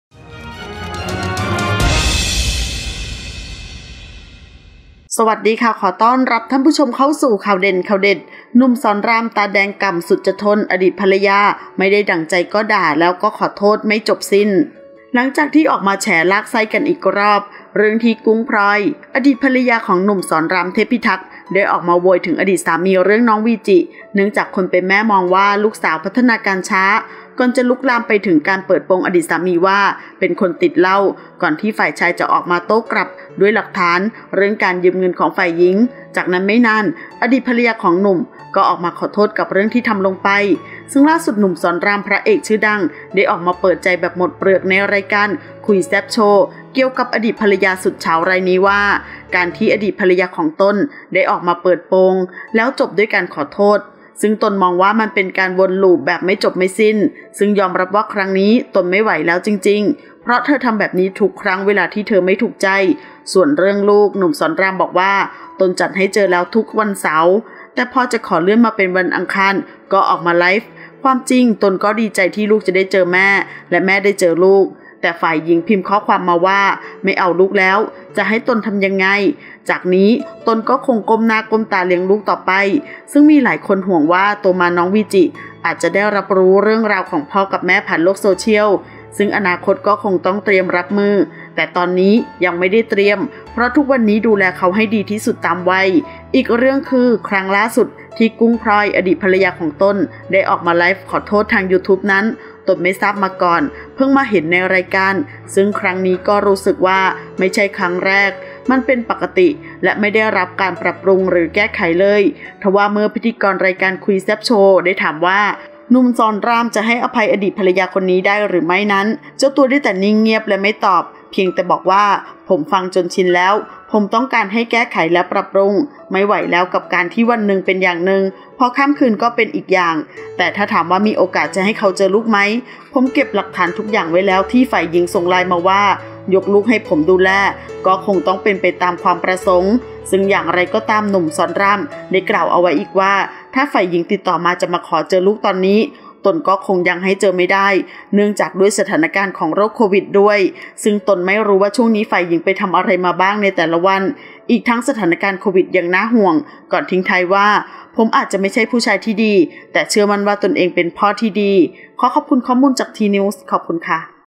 หนุ่ม ศรราม ตาแดงก่ำสุดจะทน อดีตภรรยา ไม่ได้ดั่งใจก็ด่าแล้วขอโทษไม่จบสิ้น
ขอบคุณที่มา :
ขอขอบคุณข้อมูลจาก: